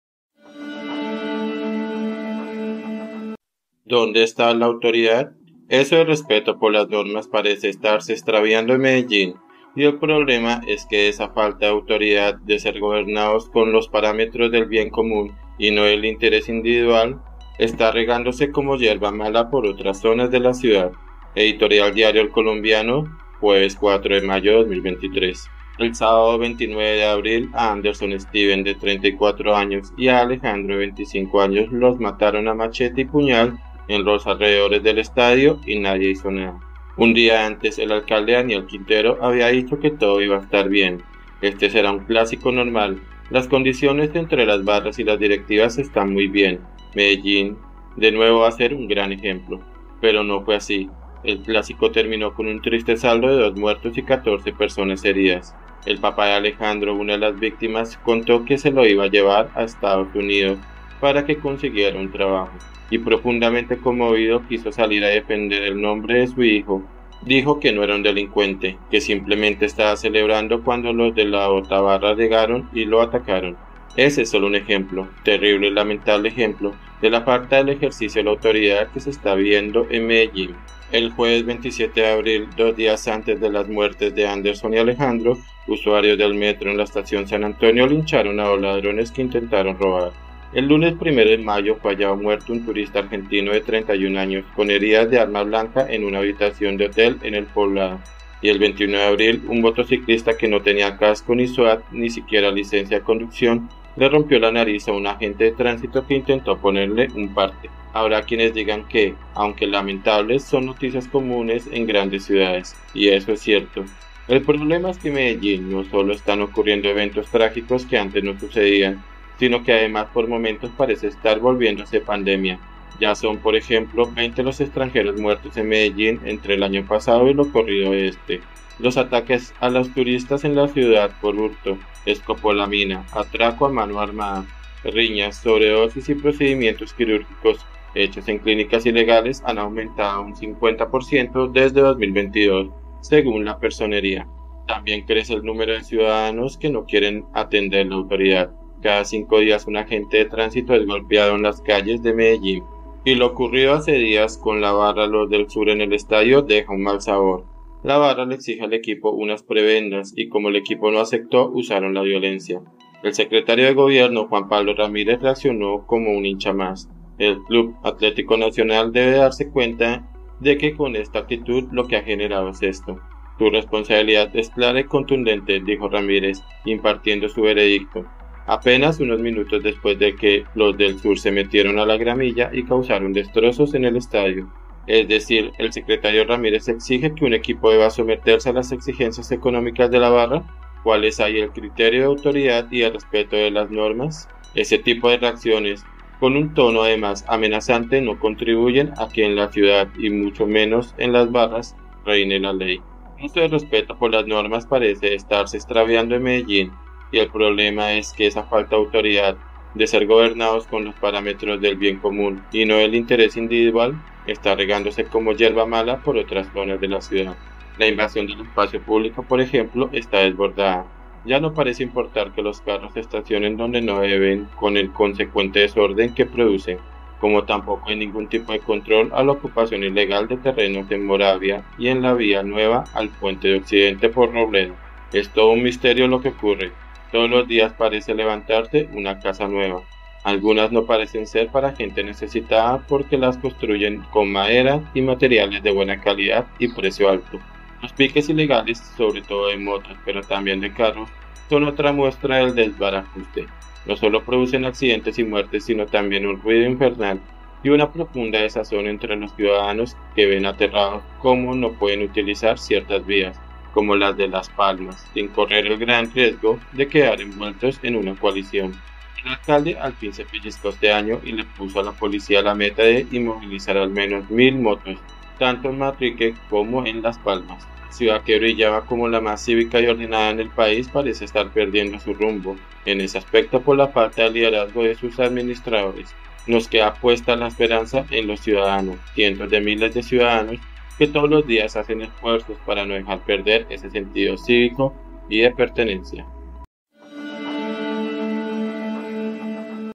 ¿DÓNDE ESTÁ LA AUTORIDAD? ⛔ Editorial El Colombiano
La ciudad que brillaba como la más cívica y ordenada en el país parece estar perdiendo su rumbo en ese aspecto por la falta de liderazgo de sus administradores. Nos queda puesta la esperanza en los ciudadanos, cientos de miles de ciudadanos, que todos los días hacen esfuerzos para no dejar perder ese sentido cívico y de pertenencia.